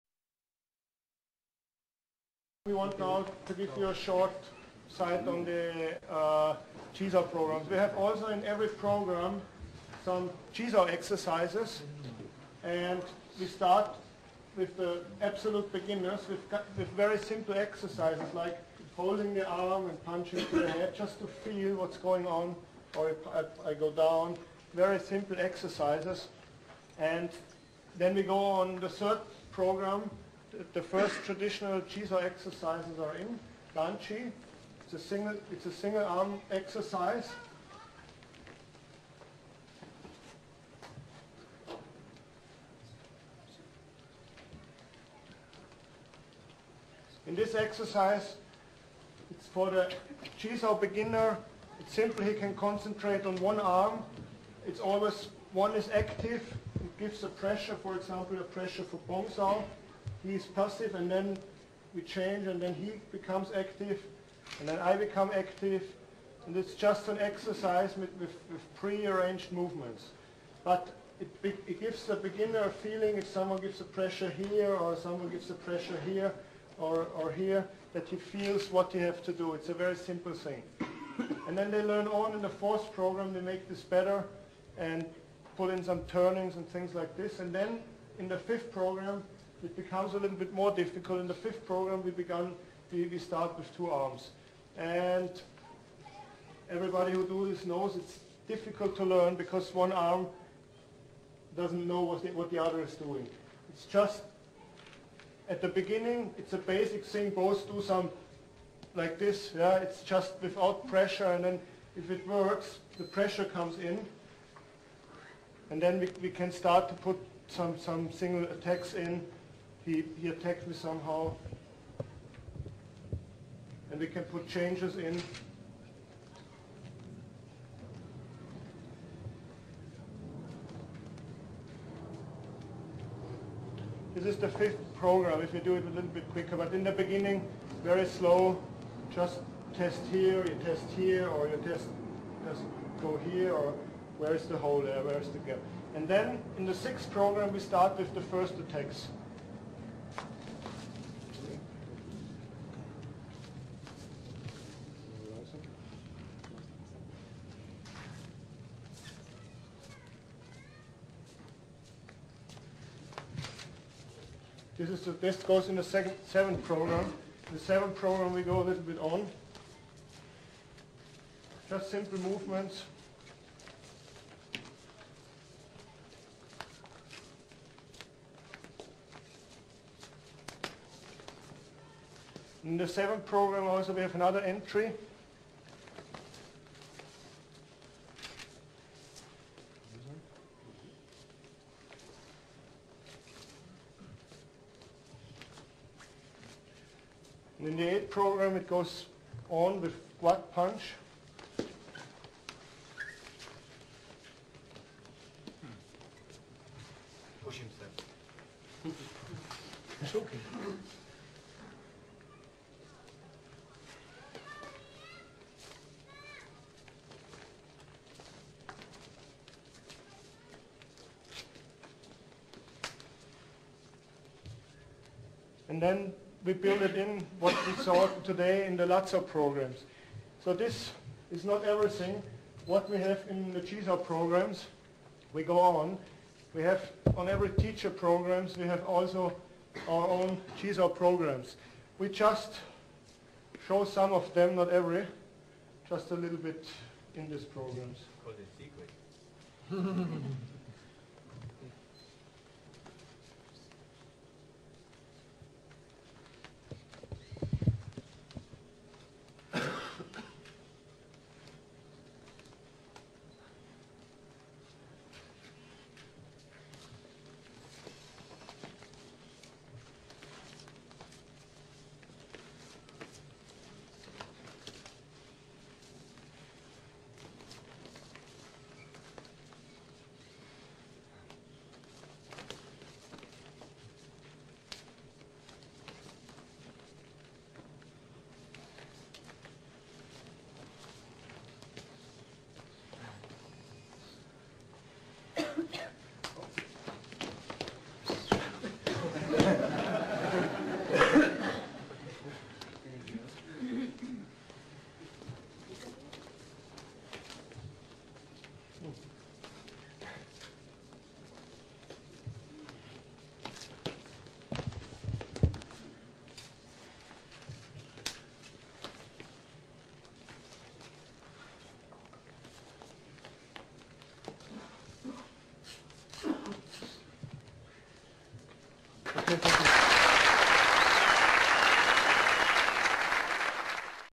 GM Konig & GM Schembri - Exhibición de Chi Sao - Subtitulos Español
Dos grandes maestros del Wing Chun de la EWTO realizando una exhibición de Chi Sao